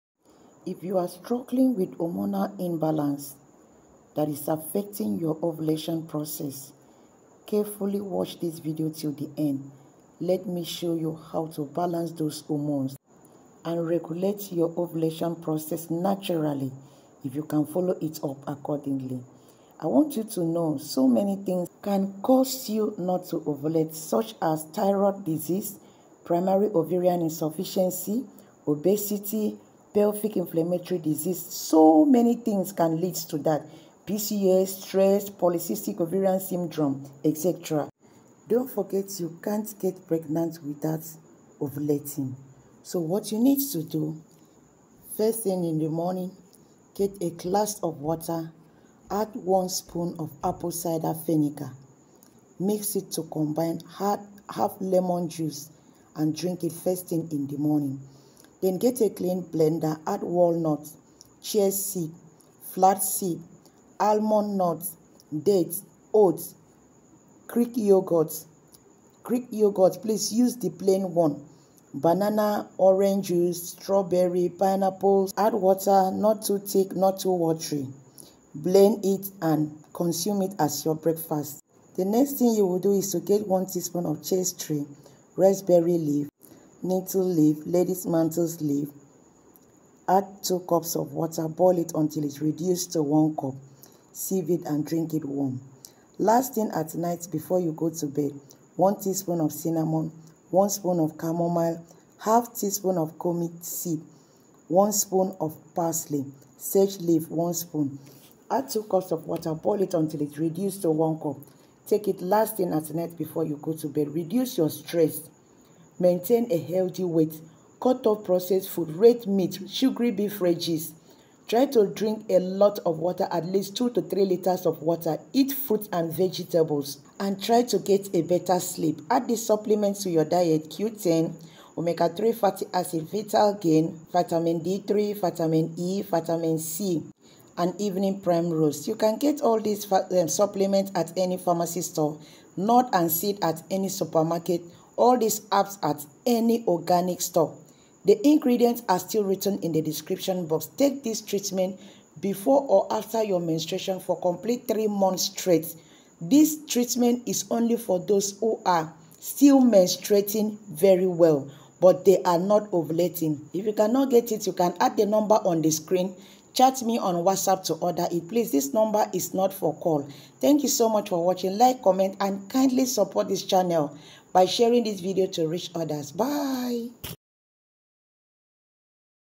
Natural Remedy To Balance Hormones And Regulate Periods Naturally
How to balance hormones and regulate ovulation naturally at home.

Ingredients
Smoothie
Walnuts
Chia seeds
Flaxseed
Almond nut
Dates
Oats
Greek yogurt plain
Banana, orange, berries, pineapple

After breakfast
Chaste tree
Raspberry leaf
Stinging nettle leaf
Lady's mantle

Night
Cinnamon
Chamomile
Clove
Cumin
Parsley
Sega leave

Organic castor oil

Supliements
Vitamin E
Cq10, vitamin D3, vitamin C, folic acid, omega 3 fatty acids, fertilegain, evening primrose